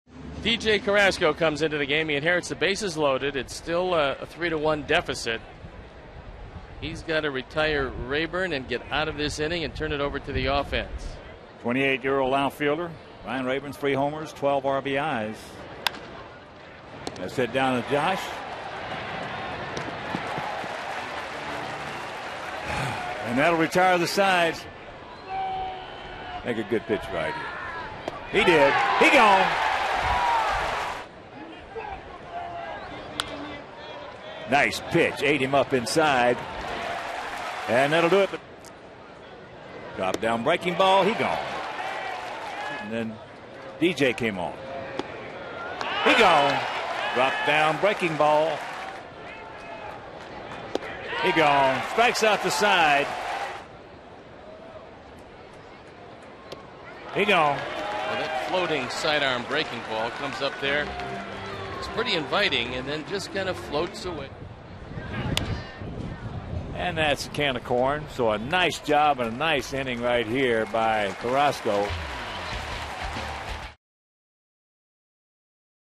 2009/06/08 Carrasco strikes out five
DET@CWS: Carrasco collects five K's in 3 1/3 innings